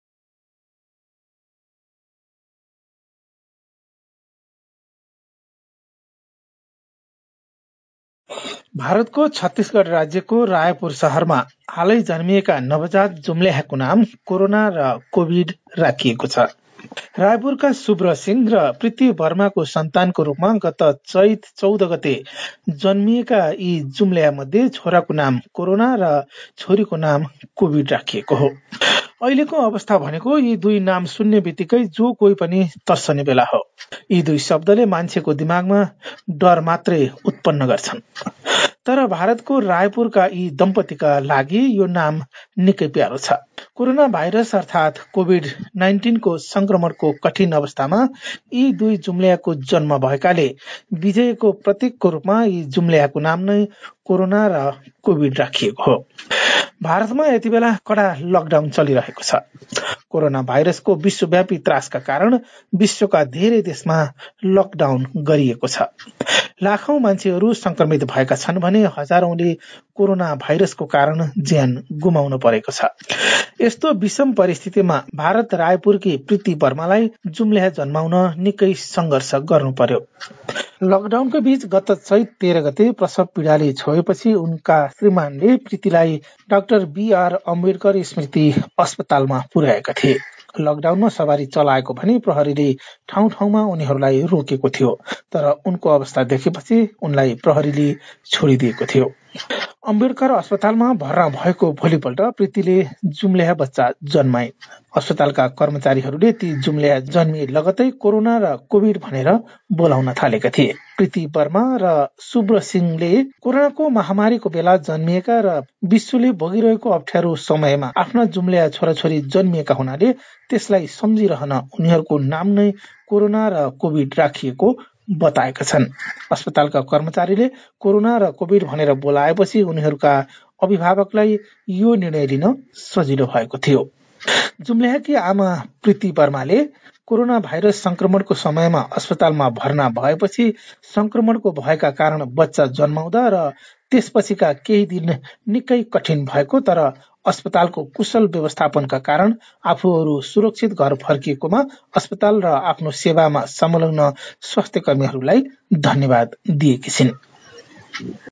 काेराेना महामारीमा जन्मेका जुम्ल्याहकाे नाम काेराेना र काेभिड राखियाे ।
भारतको छत्तीसगढ राज्यकाे रायपुर शहरमा हालै जन्मिएका नवजात जुम्ल्याहाके नाम काेराेना र केभिड राखिएके छ ।
रायपुरका शुभ्र सिंह र प्रिती बर्माका सन्तानकाे रूपमा गत चैत १४ गते जन्मिएका यी जुम्ल्याह मध्ये छेराकाे नाम ‘कोरोना’ र छेरीकाे नाम ‘कोभिड’ राखिएको छ ।
अहिलके अवस्थामा काेराेना र काेभिड दुइ नाम सुन्ने वितिकै जे केही पनि तर्सने बेला हाे । यी दुई शब्दले मान्छेके दिमागमा डर उत्पन्न गर्छन्। तर भारतके रायपुरका दम्पतीका लागि अहिले याे नाम निकै प्याराे छ । केरेना भाइरस अर्थात केभिड १९ काे संक्रमणकाे कठिन अवस्थामा यी दुइ जुम्ल्याहके जन्म भएकाले विजयको प्रतीकके रूपमा यी जुम्ल्याहाके नाम केराेना र केभिड राखिएके हाे।
जन्मने वित्तिकै अस्पतालका कर्मचारीले जुम्ल्याहकाे नाम काेराेना र काेभिड भनेर बाेलाएका थिए । याे नाम उनीहरूका अभिभावकलाइ पनि ठीकै लाग्याे र नामाकरण गरियाे काेराेना र काेभिड ।